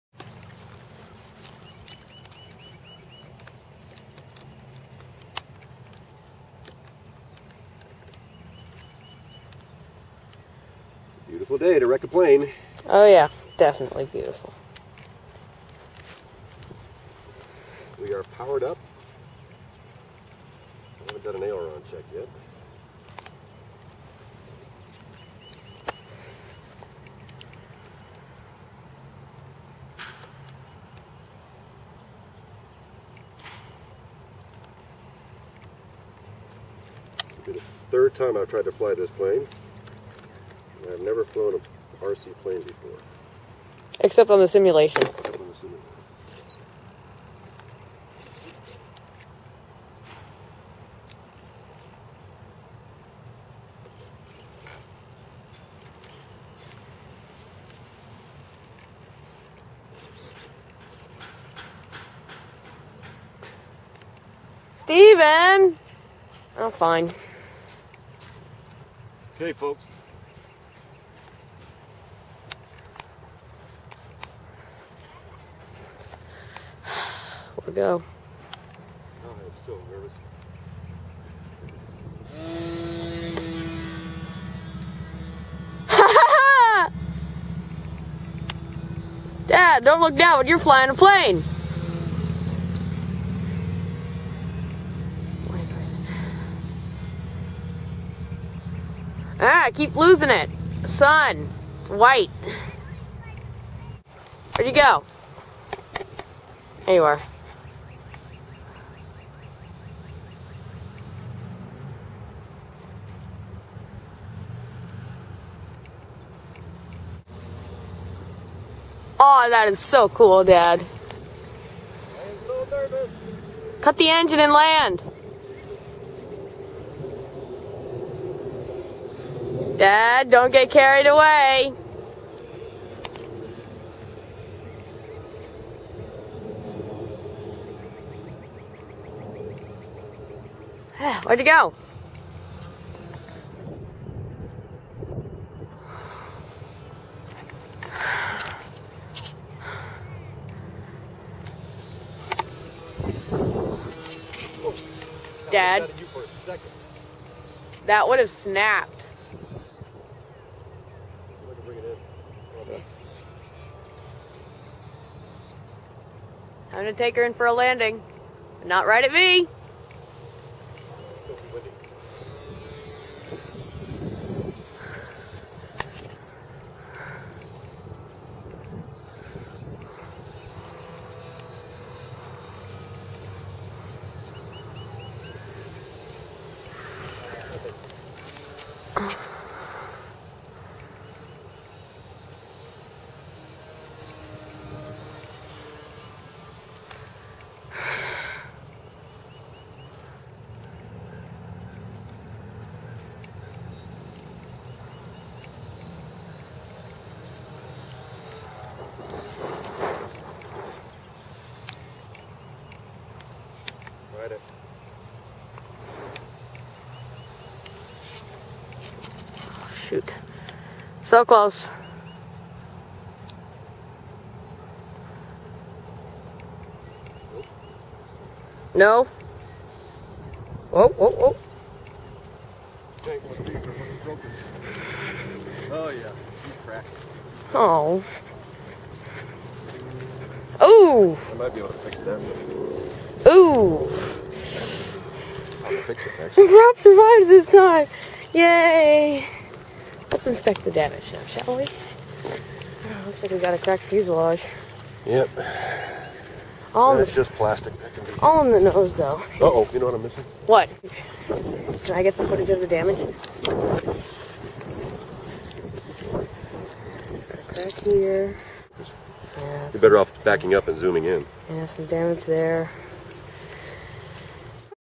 My First RC Flight
Third try at flying this OLD Omei 2000 given to me as a kit by a friend.